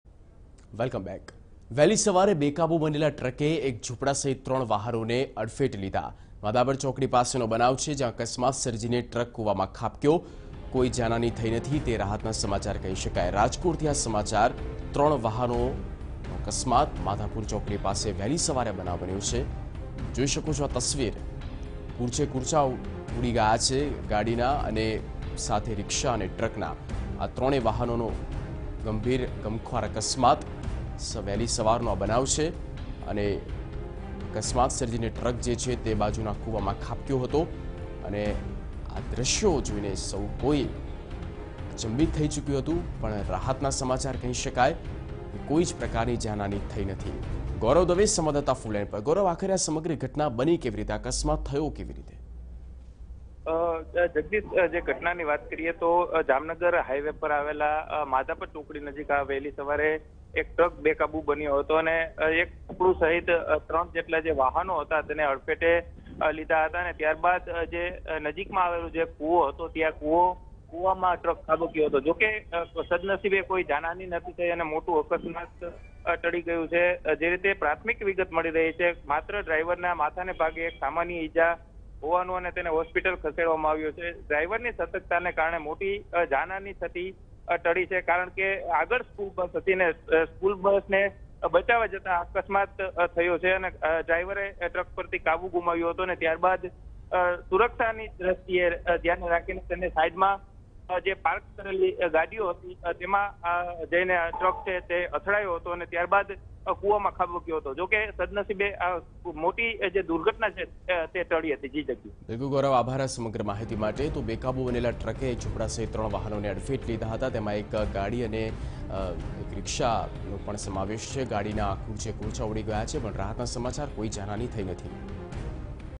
Rajkot: Truck falls into well near Madhapar cross road, no causalities reported- Tv9
Rajkot: Truck falls into well near Madhapar cross road, no causalities reported